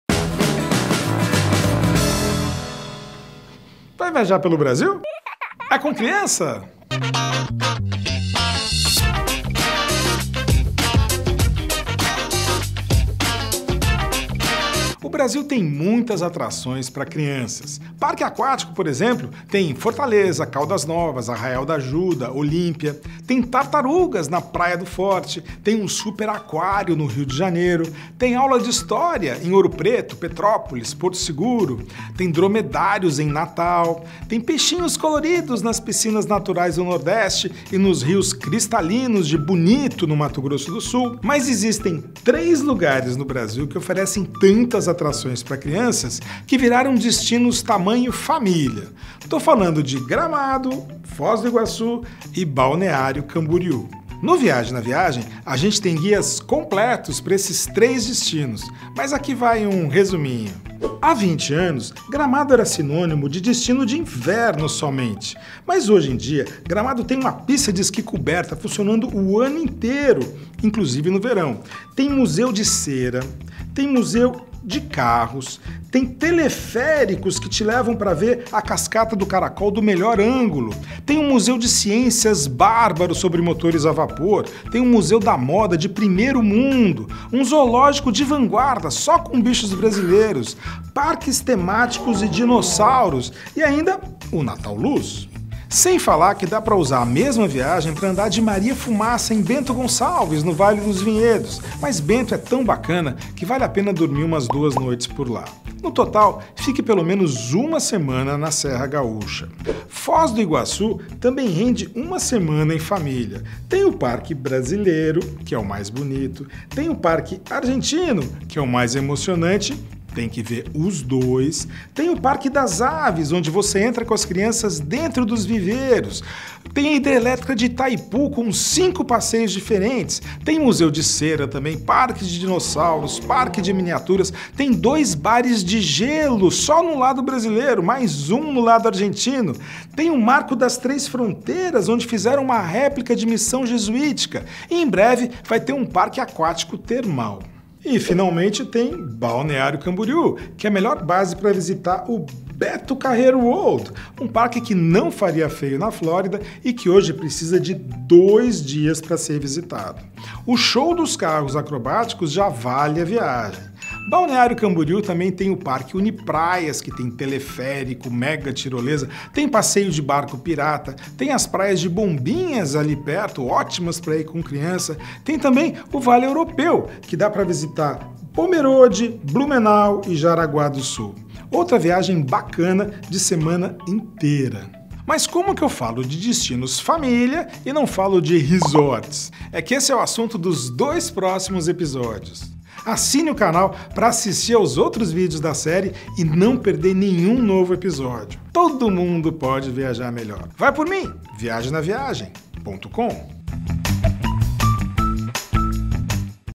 Destinos família - Vai por mim Brasil - Ricardo Freire
O Brasil tem muitas atrações pra crianças. Parque aquático, por exemplo, tem em Fortaleza, Caldas Novas, Arraial d’Ajuda, Olímpia.

Tem tartarugas na Praia do Forte. Tem um super aquário no Rio. Tem aula de história em Ouro Preto, Petrópolis, Porto Seguro. Dromedários em Natal. Peixinhos coloridos nas piscinas naturais do Nordeste e em Bonito.

Mas existem três lugares no Brasil que oferecem tantas atrações pra crianças que viraram destinosparafamília - Gramado, Foz do Iguaçu e Balneário Camboriú. No Viaje na Viagem a gente tem guias completos para viagem aos três destinos e o Ricardo Freire faz um resumo nesse vídeo.

Créditos:
Roteiro e apresentação: Ricardo Freire
Produção executiva: Elisa Araujo
Direção: Pedro Palmeiro
Câmera: Rafael Azeredov
Áudio, Edição e Motion Graphics: Geovane Brelaz
Assistente de estúdio: Adriano Santos e Júlio César Cantini
Vinheta: Rebellmotion
Fotos Mini-me: Marco Ferraz
Agradecimentos: Nick Santiago, Maria Clara Jorge e Studioline
Realização: Viaje na Viagem e H+ Conteúdo